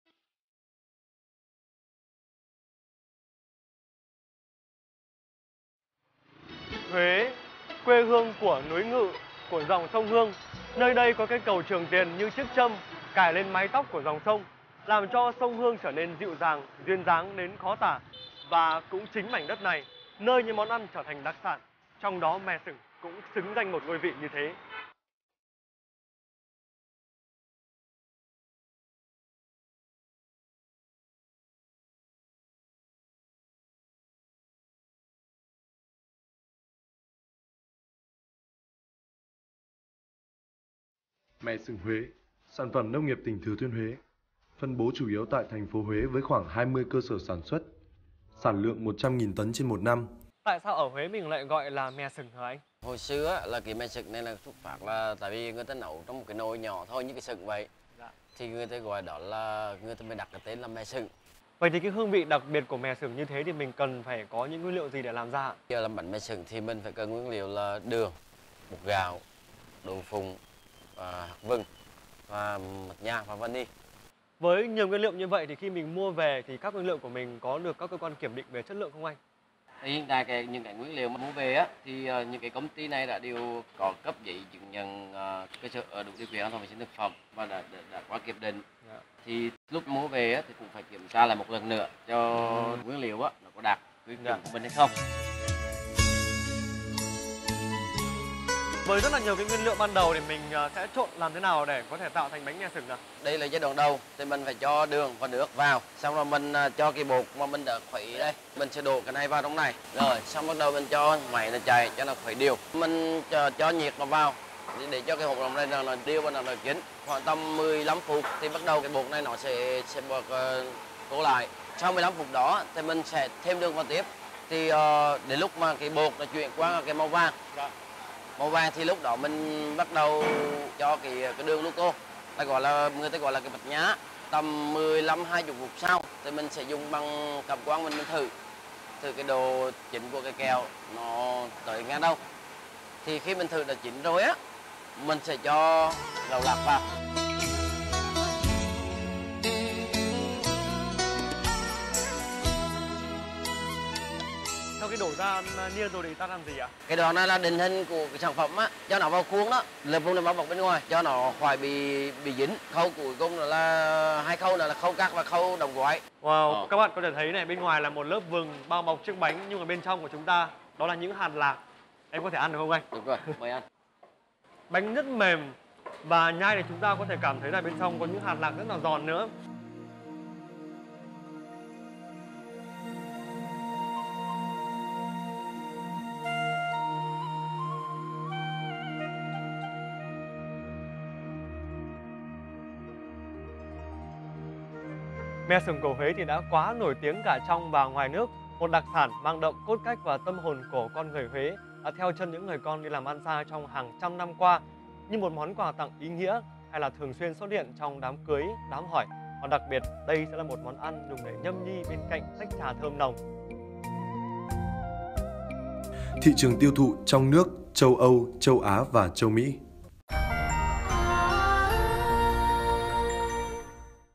Nông nghiệp sạch số 176 - Mè xửng Huế
Từ lâu, mè xửng là niềm tự hào của tất cả người dân Huế. Món ăn này có màu vàng, trong suốt, bóp hoặc bẻ thấy mềm nhẹ nhưng không gãy, thả tay ra lại trở về hình dạng cũ; khi ăn dễ dùng, thơm hương vị đậu mè, mạch nha quyến rũ.

Một thứ kẹo ngon lại còn dai dai, ăn hoài vẫn không thấy ngán, kẹo mè xửng níu kéo cũng chính từ sự dẻo dai này làm lưu luyến bao người mê kẹo.